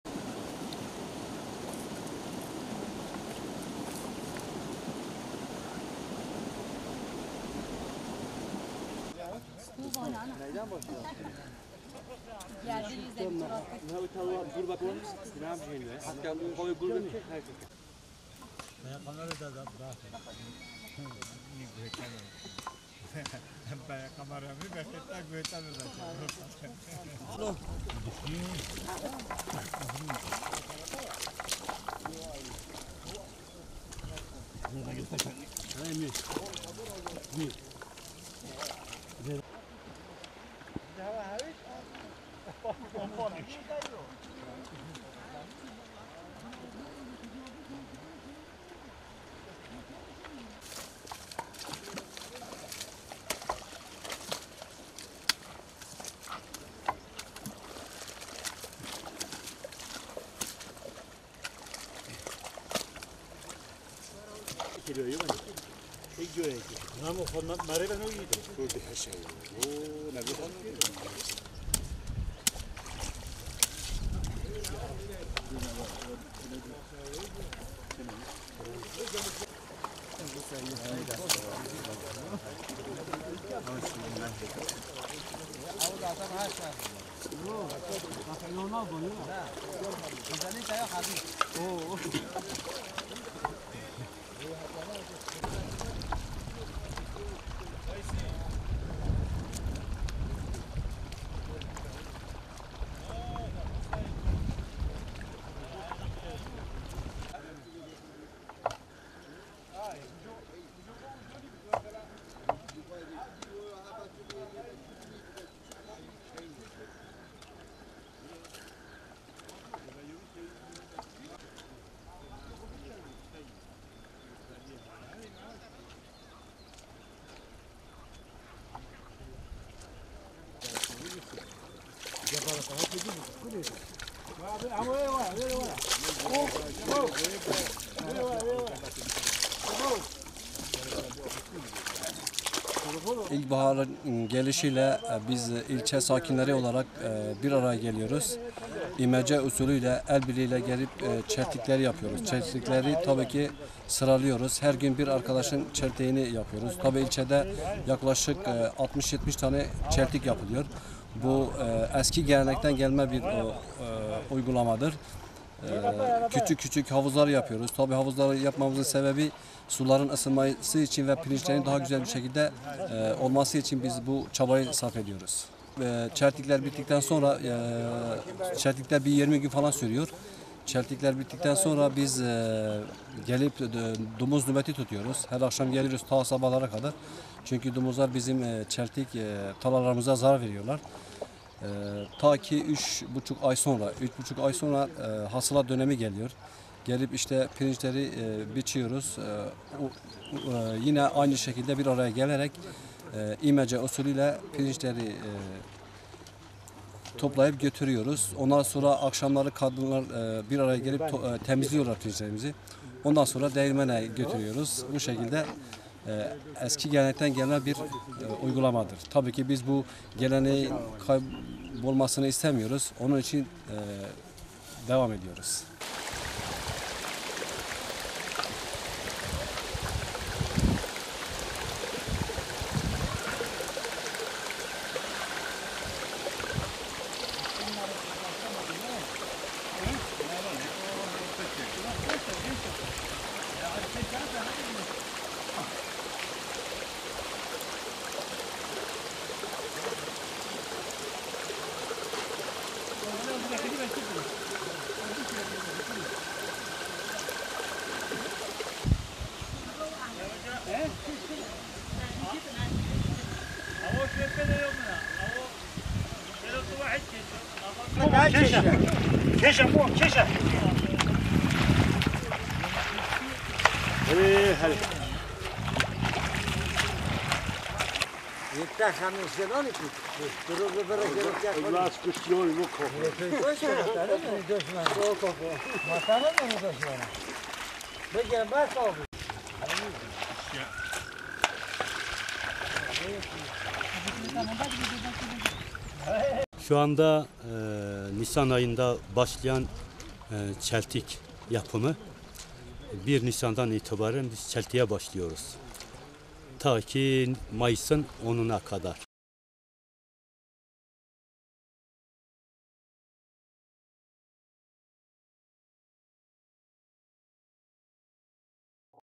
Çukurca'da imece usulü çeltik ekiliyor
Hakkari'nin Irak sınırındaki Çukurca ilçesine bağlı Yeşil çeşme mahallesinde vatandaşlar, arazi şartları nedeniyle tarım makinelerinin giremediği tarlalarda imece usulüyle ekim yapıyor.